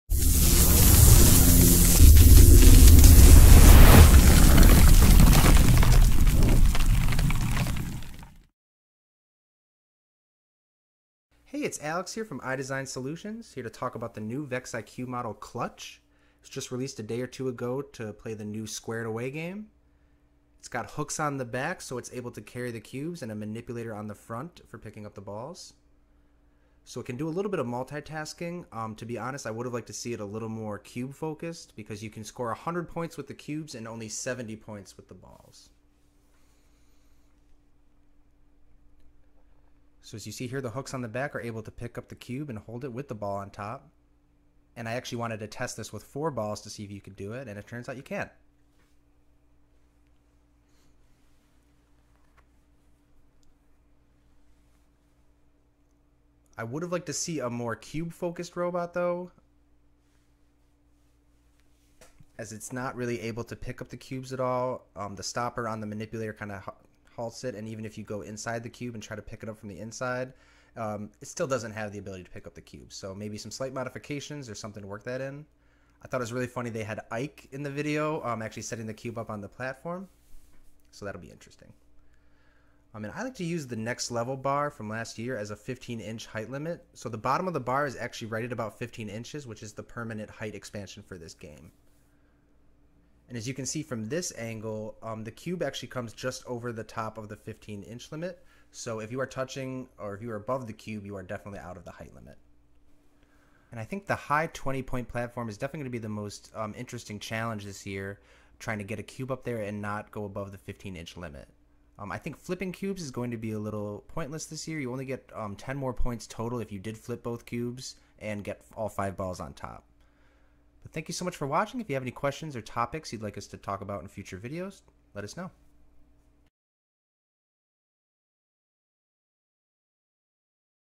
VEX IQ New Squared Away Model Clutch
Check out the new design for VEX IQ that can be used for the Squared Away game.  This explains things to consider and prioritize when using this build to compete. It also illustrates that it meets the 15" threshold. You will need the VEX IQ Super Kit to build Clutch.